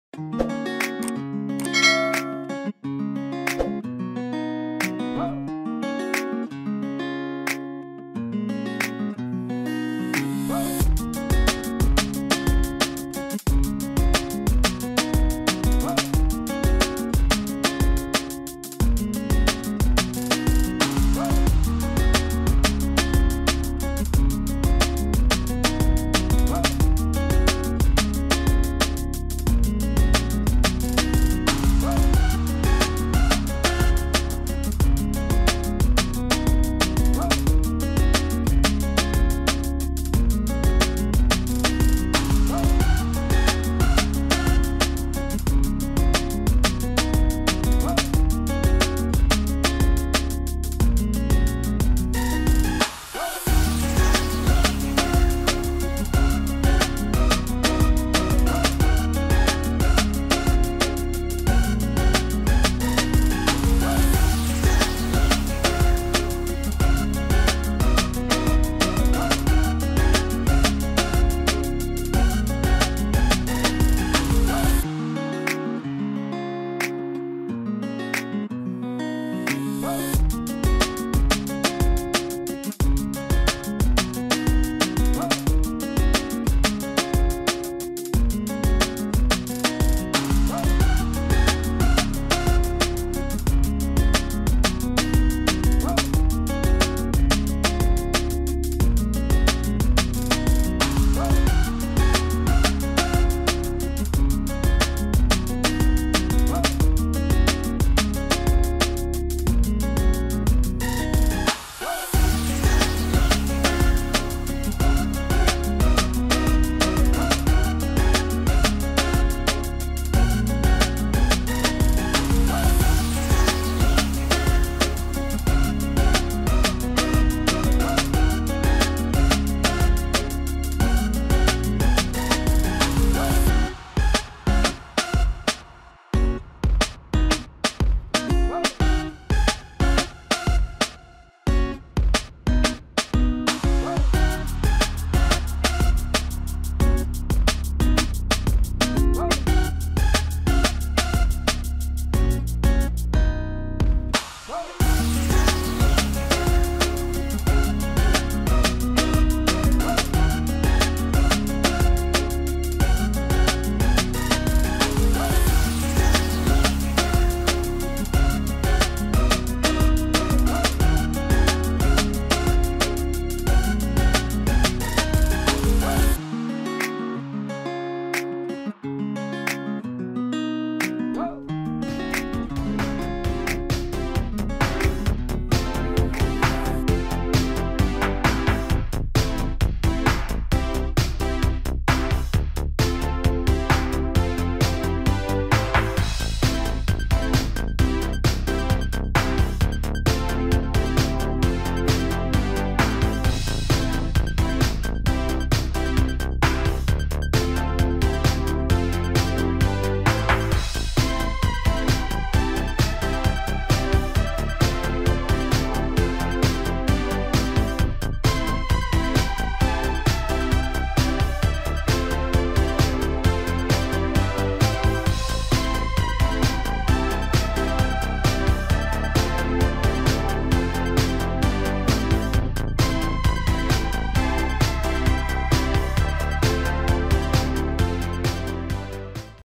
Как нарисоать LOOKIES из RAINBOW FRIENDS Chapter 2 правильно и неправильно
Всем привет, сегодня я покажу как нарисовать LOOKIES нового монстра из RAINBOW FRIENDS Chapter 2 правильно и неправильно. Давайте рисовать вместе со мной на канале Рисунки Юльки.